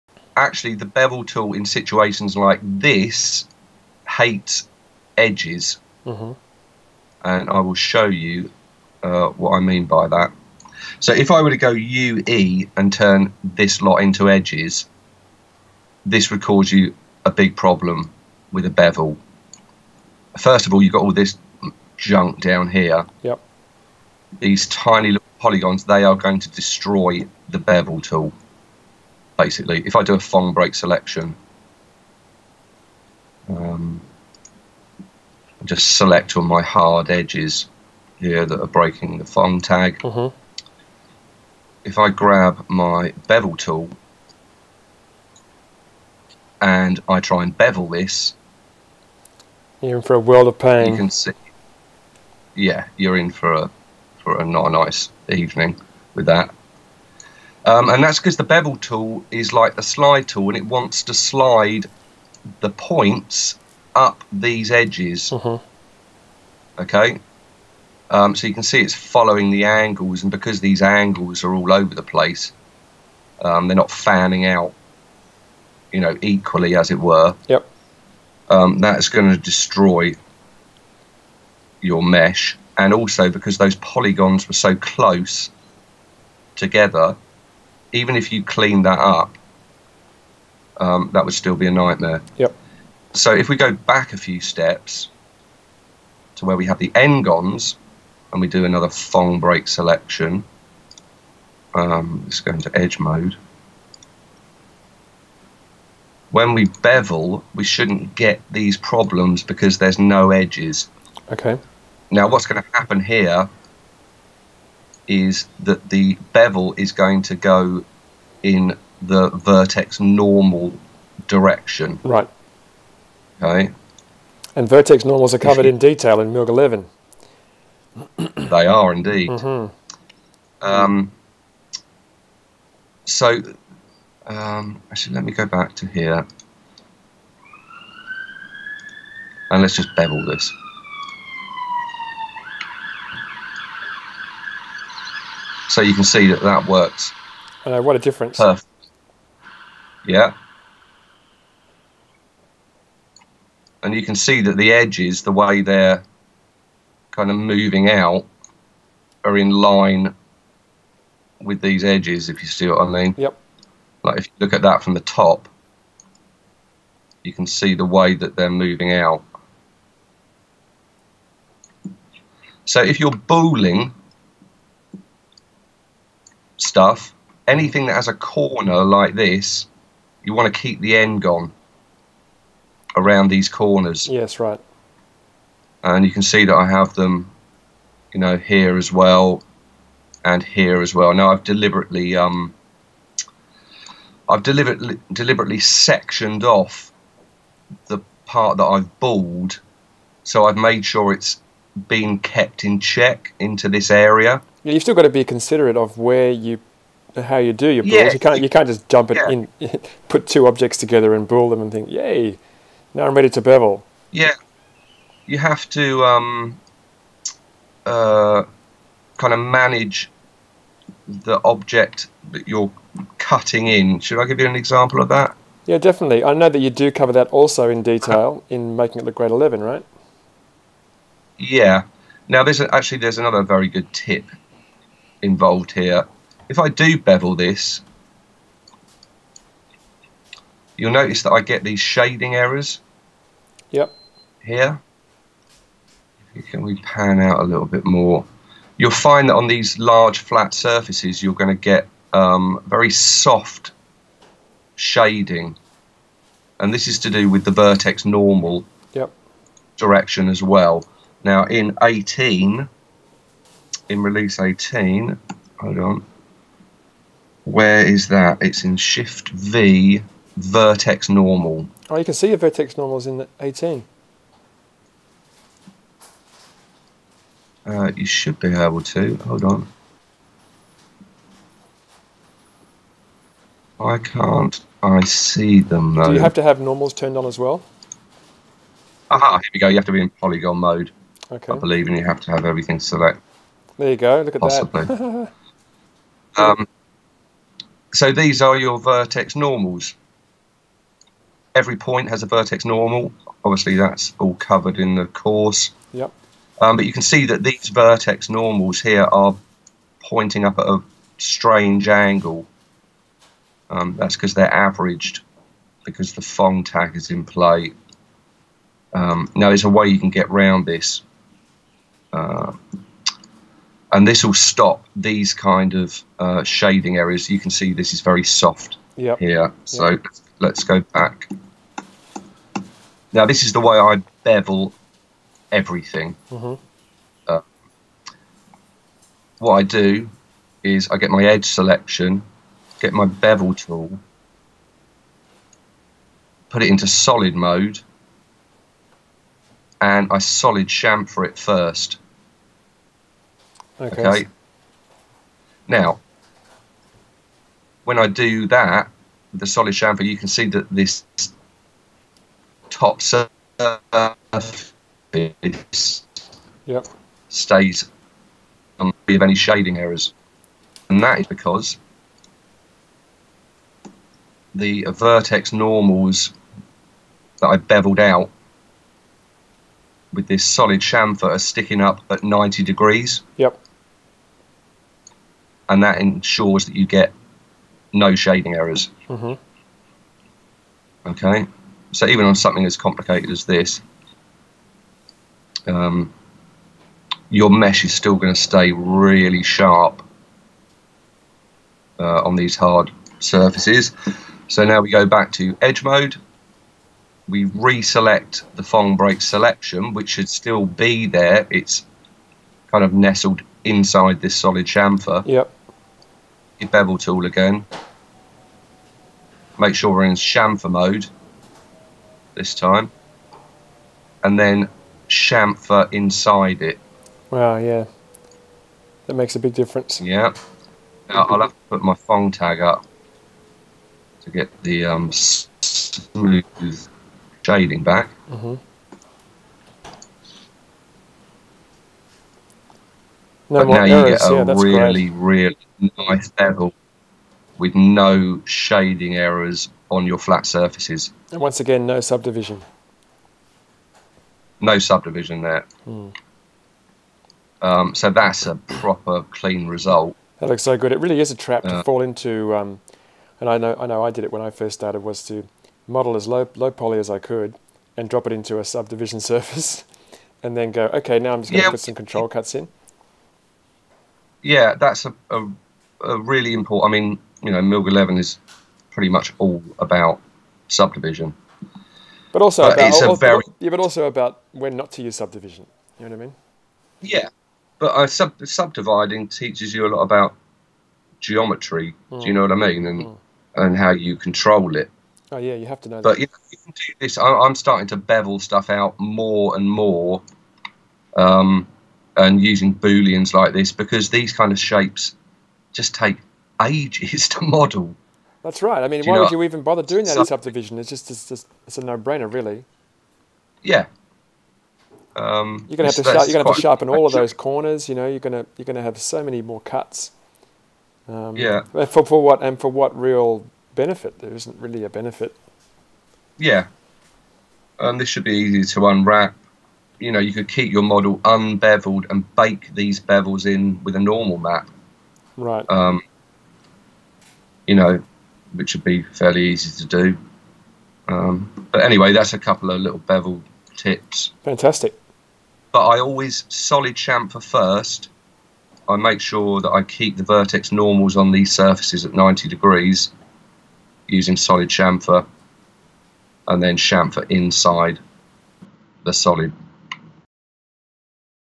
Bevelling Booles in Cinema 4D
A short skype recording in which Toby Pitman demonstrates a technique for getting clean bevels from booled objects in Cinema 4D.